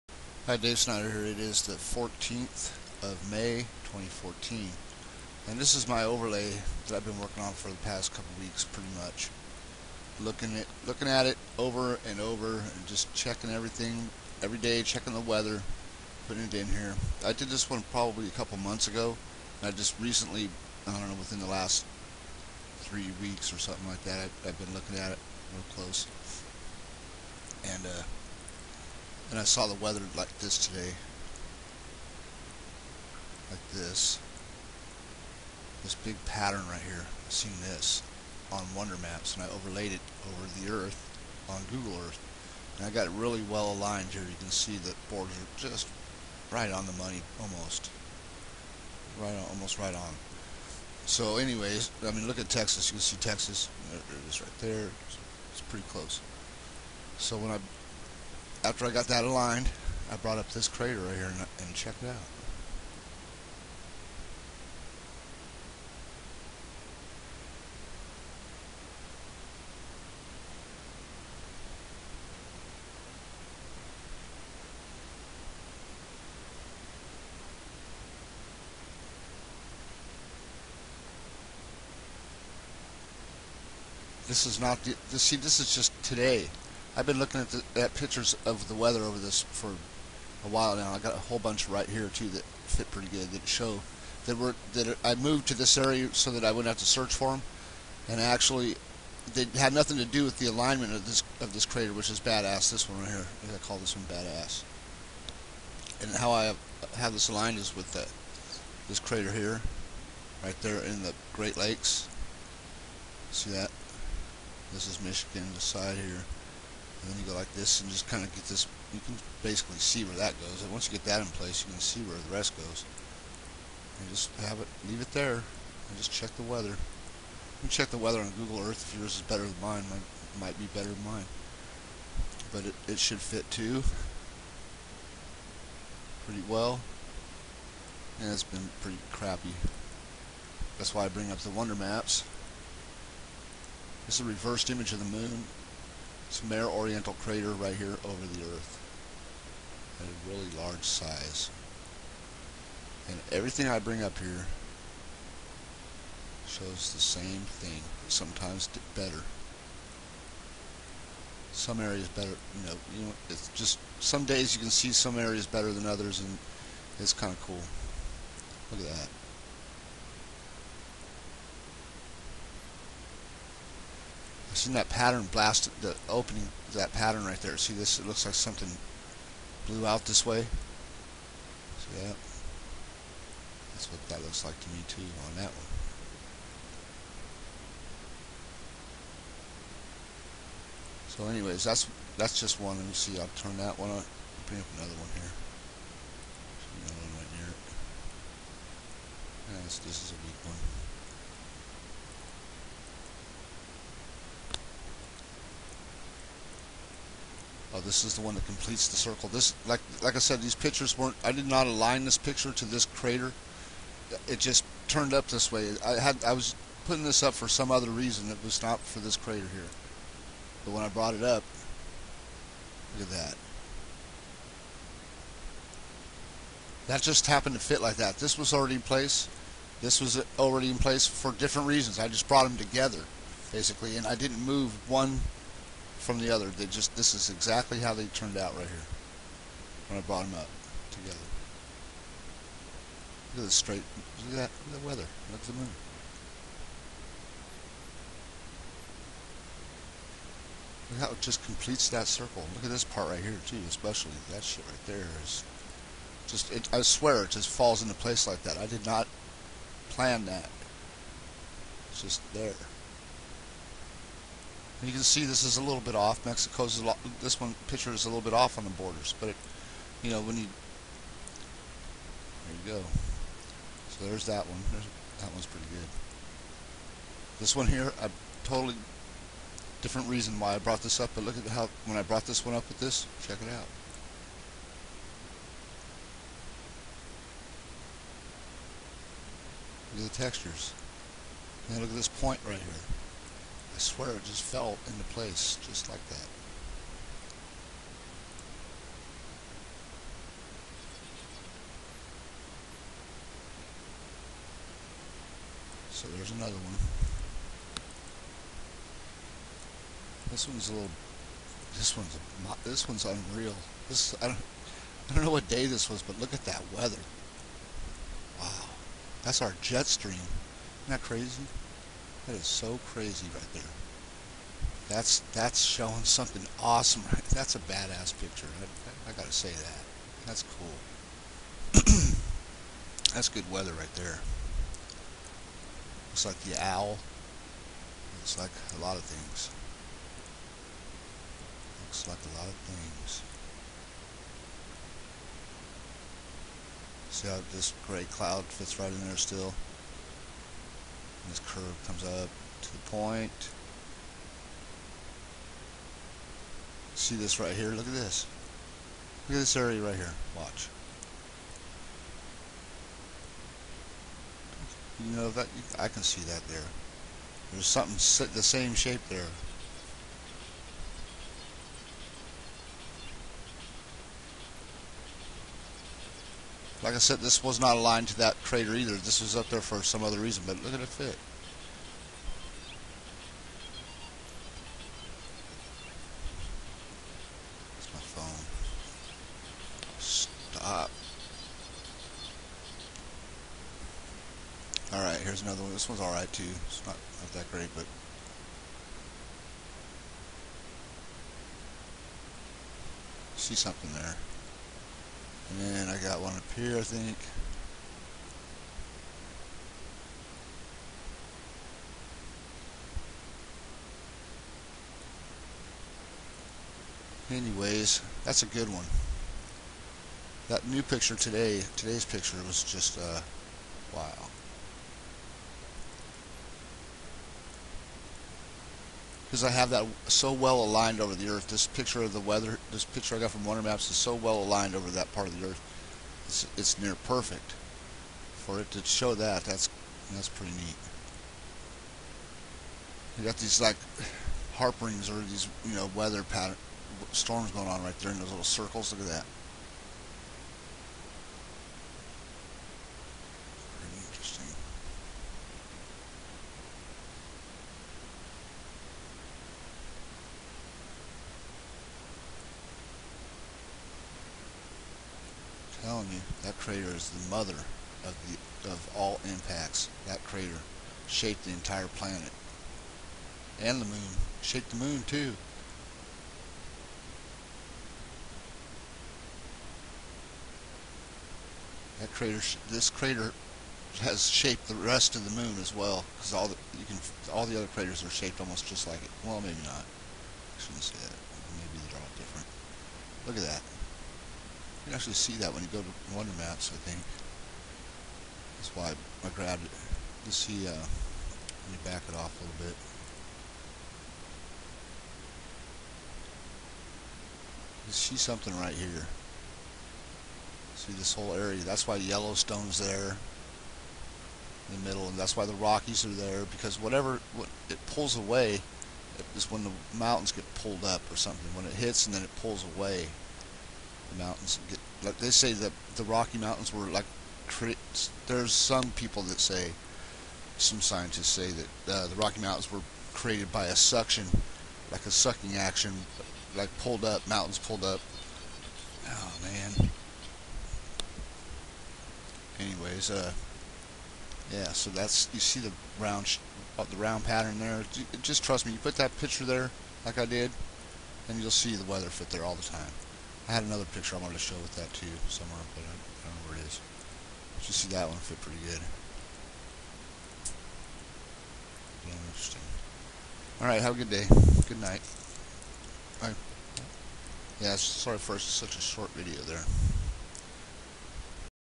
Sooner Or Later, You Are Going To See The Truth Of This Planet...
You will see that the moon is the cause of extinctions, and severe weather, just keep watching, unless you are already fulfilled with the bullshit lies you are told?